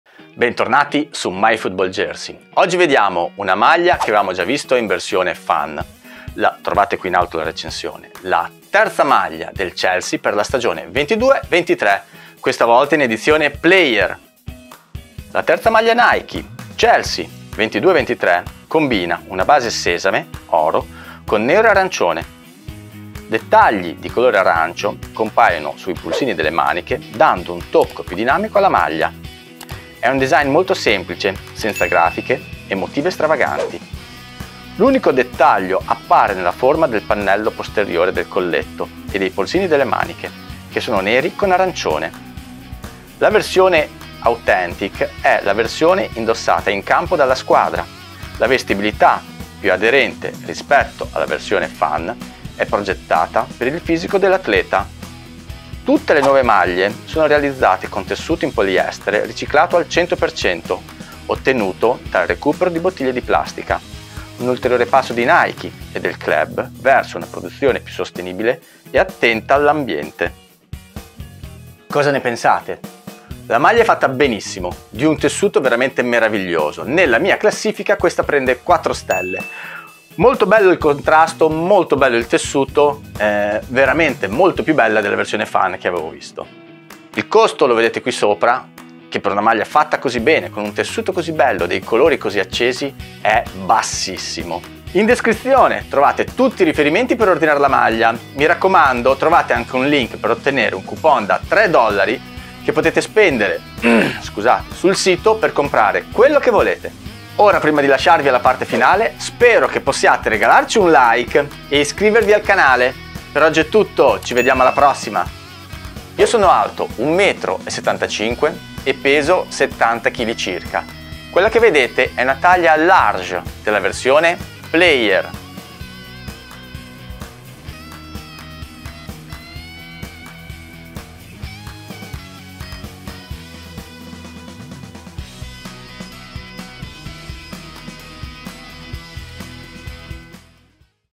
Chelsea Third Jersey 22/23 (JJSport24) Player Version Unboxing Review
Discover the 22/23 Chelsea Third Jersey (Player / Match / Authentic Version)

You can use the $ 3 coupon to buy whatever you want on JJSport24 included the Chelsea Third Jersey 22/23.

🕘 Timecodes
00:00 Overview
00:24 Details View
01:10 Ranking
01:55 Dimensions & Try On

Please watch the Chelsea Third Jersey 22/23 video review in 1080p (HD) 📺

Follow me also on TikTok @my.football.jersey to watch the Chelsea Third Jersey 22/23 video review in a different way. 💥